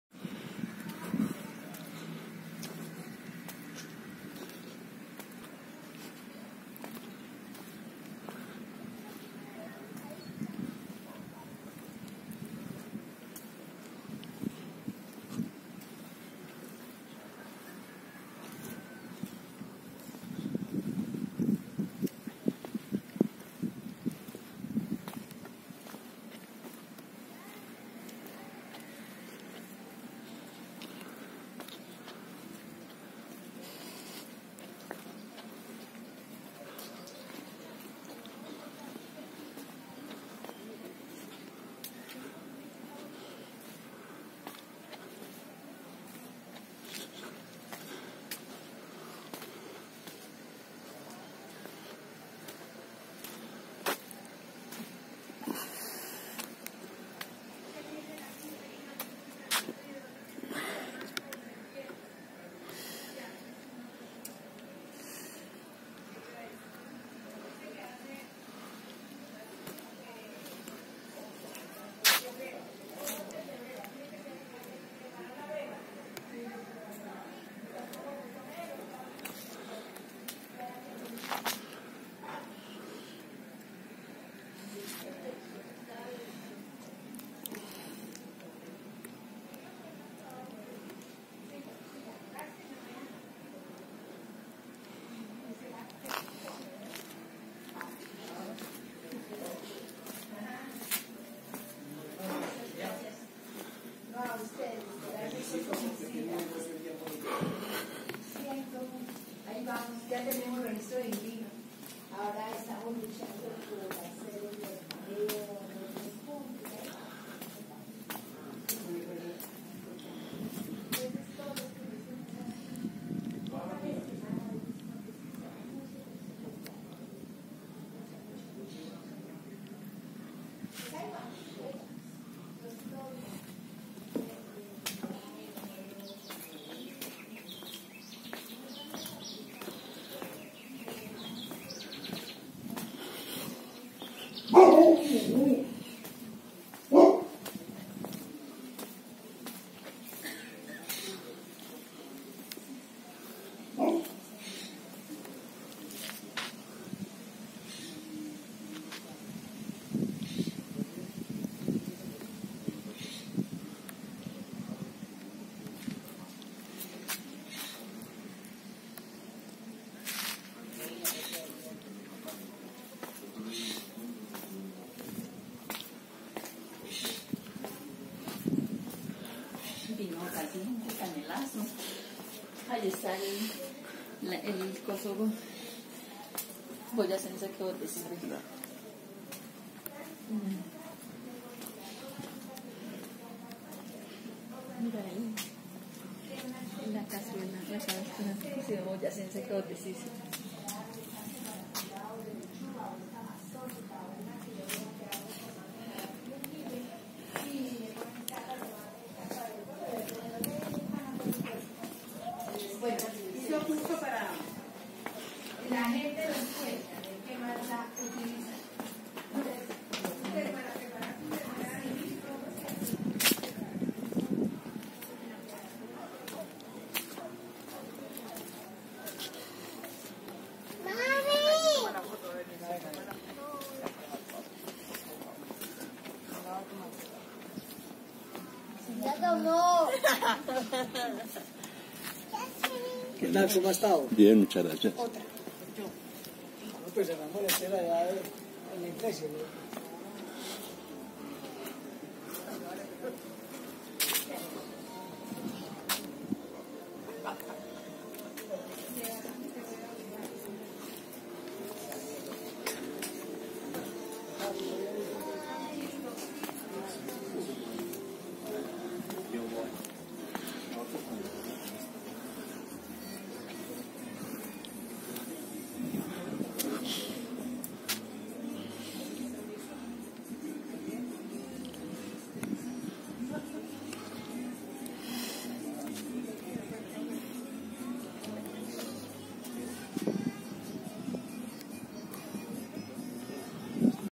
PUEBLITO BOYACENSE.
Representación de los hermosos pueblos del departamento de Boyacá .